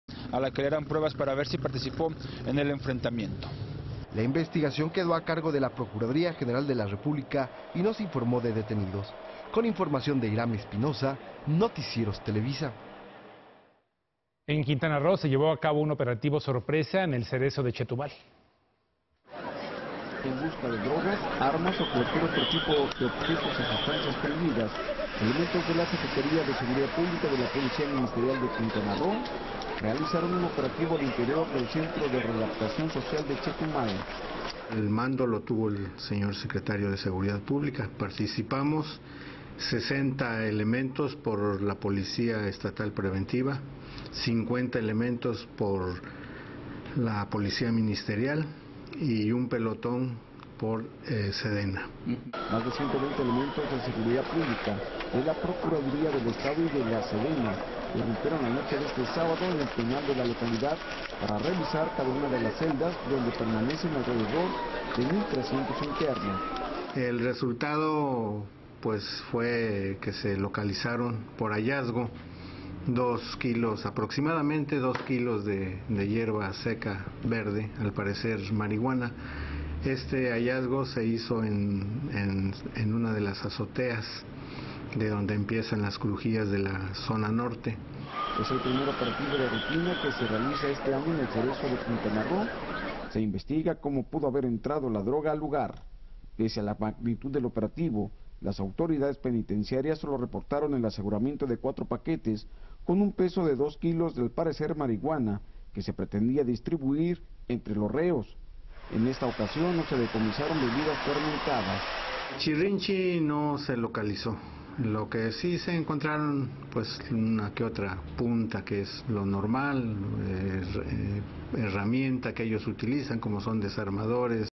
Decomisan droga en el Cereso de Chetumal
Decomisan 2 kilos de mariguana durante un operativo sorpresa en el Cereso de Chetumal en el que participaron más de 120 elementos de distintas dependencias. Visita